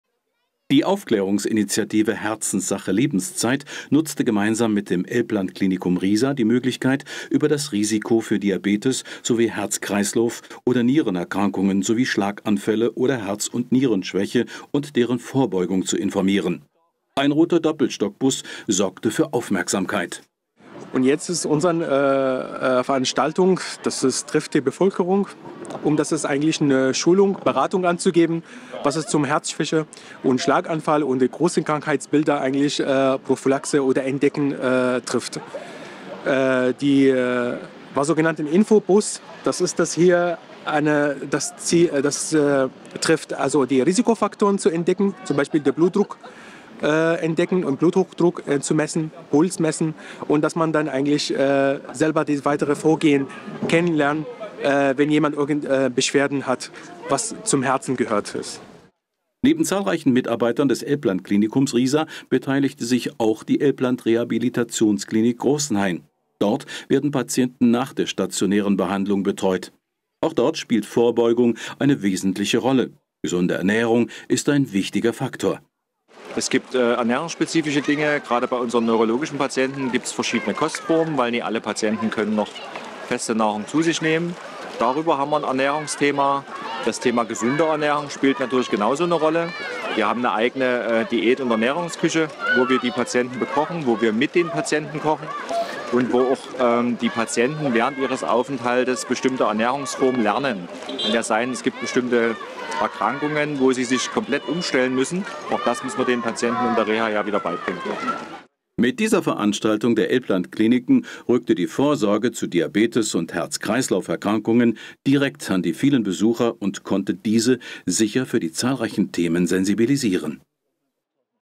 Initiative "Herzenssache Lebenszeit" - Infobus klärt mit Experten aus dem ELBLANDKLINIKUM Riesa auf
Der rote Infobus der Initiative „Herzenssache Lebenszeit“ nutzte mit unseren Experten vom ELBLANDKLINIKUM Riesa und dem SC Riesa am 12. Juni 2024 in Riesa die Gelegenheit, wichtige Aufklärungsarbeit zu leisten:

Interessierte hatten die Möglichkeit, offene Fragen zu klären und ihr persönliches Risiko für Diabetes sowie Herz-Kreislauf- oder Nierenerkrankungen wie Schlaganfälle oder Herz- und Nierenschwäche testen zu lassen.

Wir blicken auf einen erfolgreichen Tag zurück, an dem unsere Kolleginnen und Kollegen vom Klinikum wertvolle Tipps zur Vorbeugung von entsprechenden Erkrankungen übermitteln und das Risikobewusstsein fordern konnten.

VIELEN DANK!